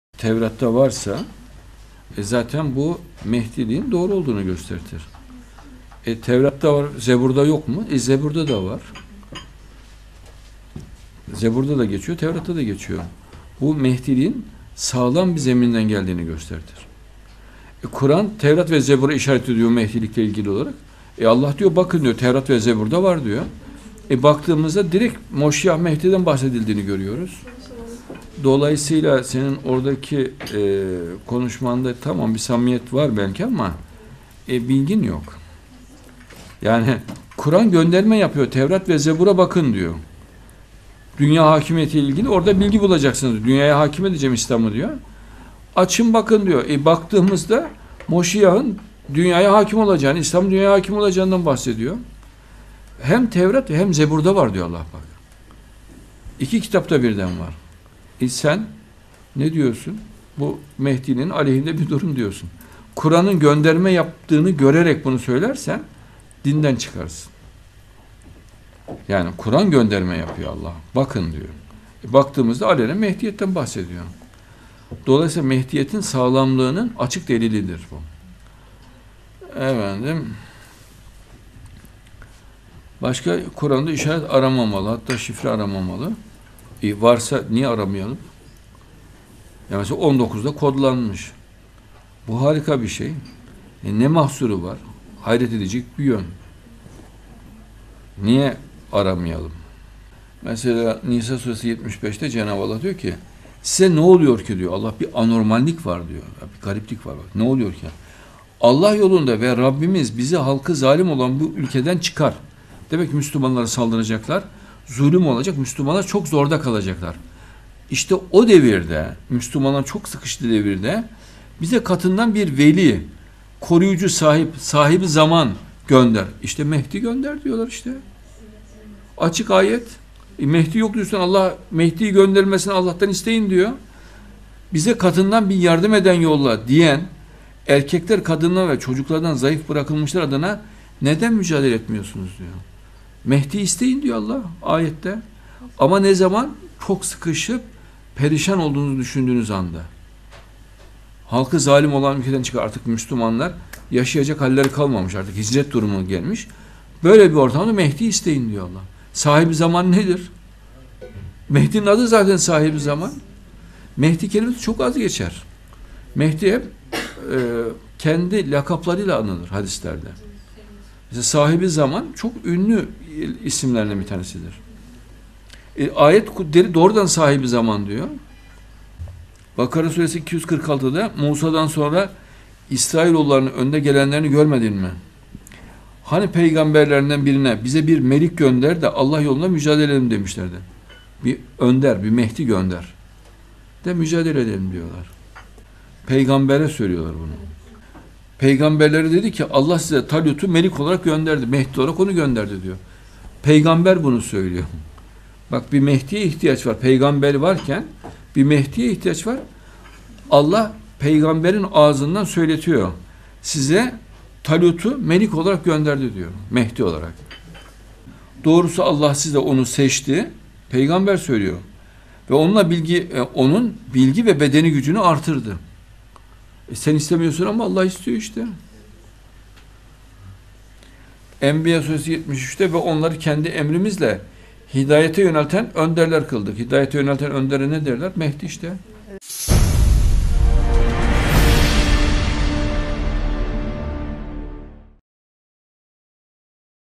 Mehdiyetin Tevrat'ta olduğunu haber veren Kuran'dır. Kuran'da...
Adnan Oktar’ın 22 Mart 2017 tarihli A9 TV röportajından